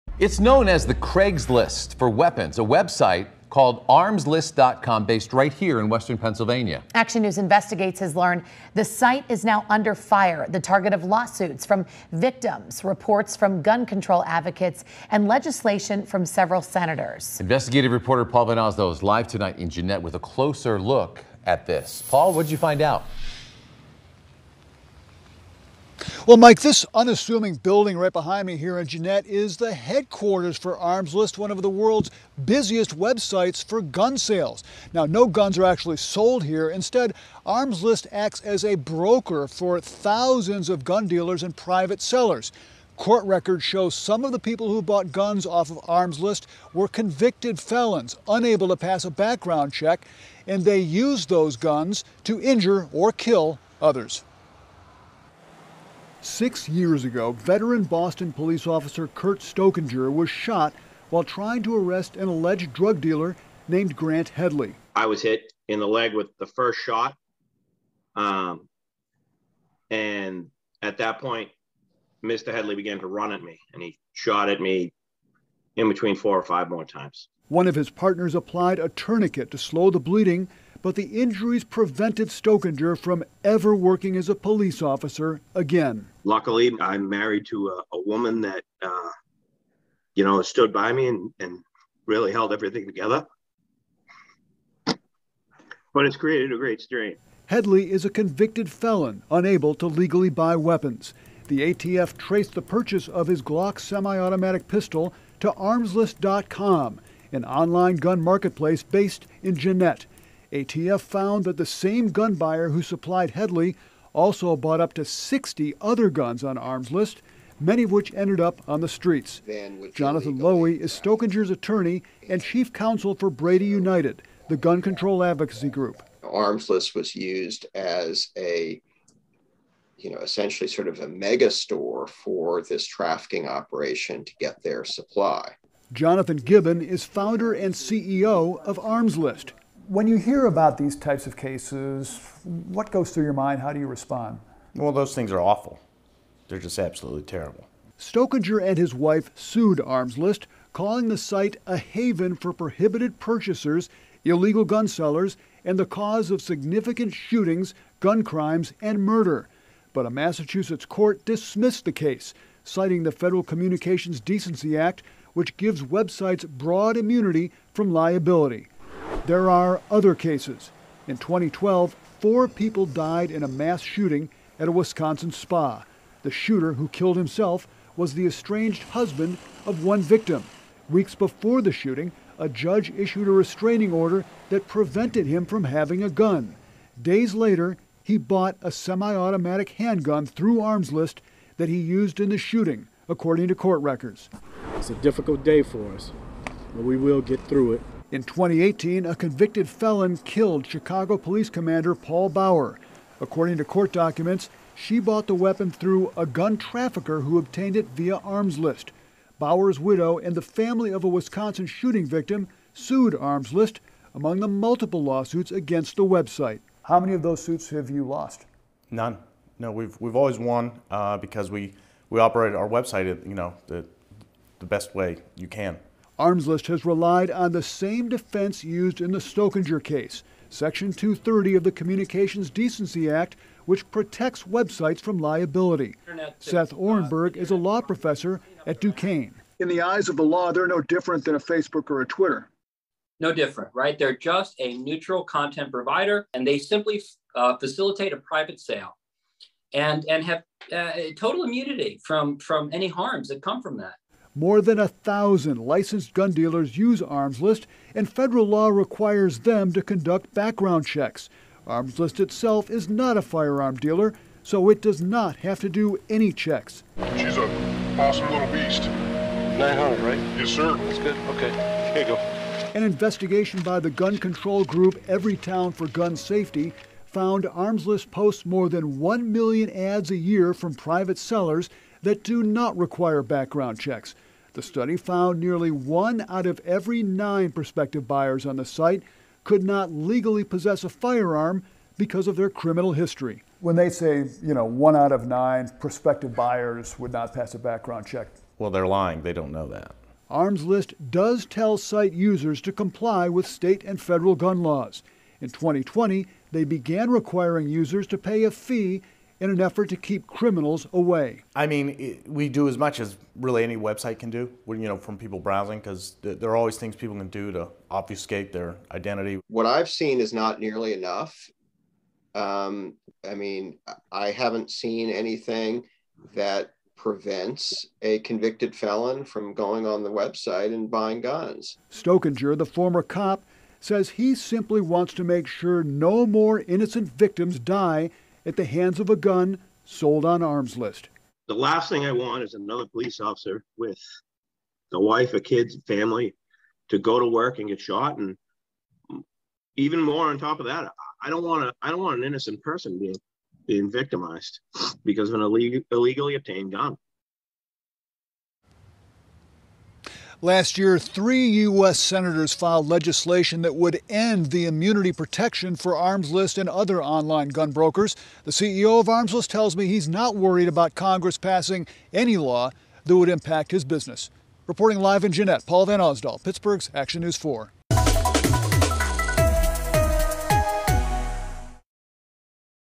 Jeannette website known as 'Craigslist for weapons' under fire from victims, gun control groups a...
Jeannette website known as 'Craigslist for weapons' under fire from victims, gun control groups and senators